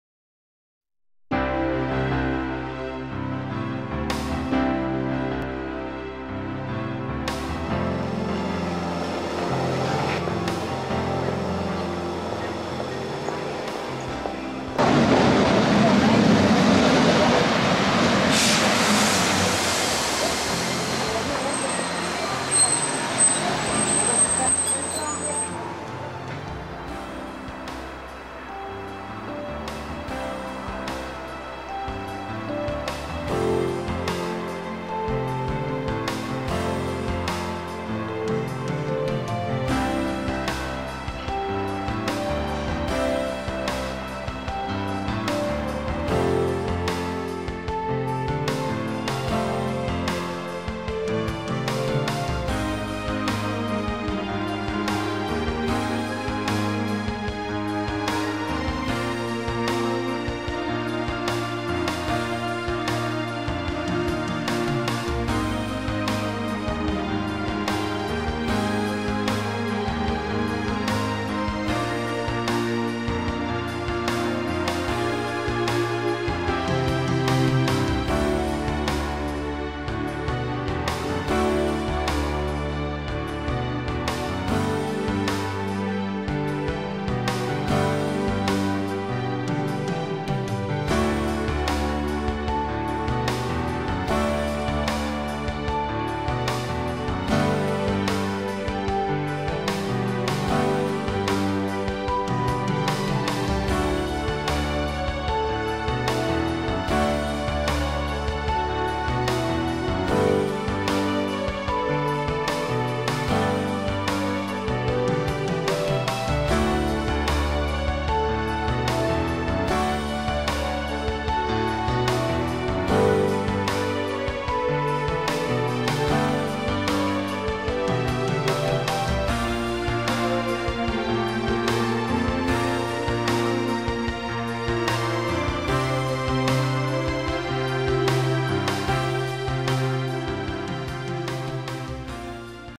富山城ライトアップ
富山城がライトアップされているので、街中ウォークを兼ねて、ホテルから散策に出かけた。